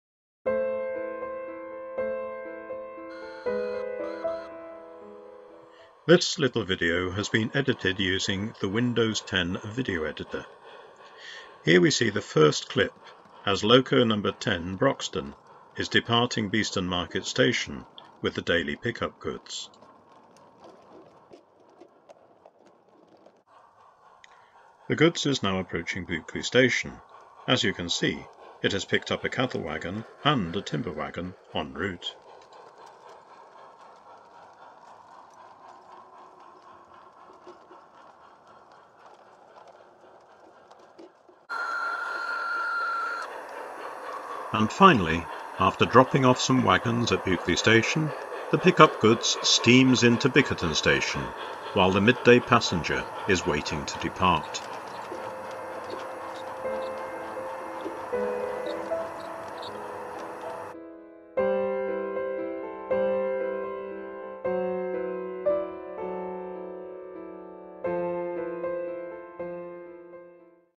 Peckforton Light Railway - Goods run - Windows Video Editor tutorial
This little video was edited using the Video Editor which comes free with Windows 10. It forms part of a tutorial which I have just written as a post on my blog.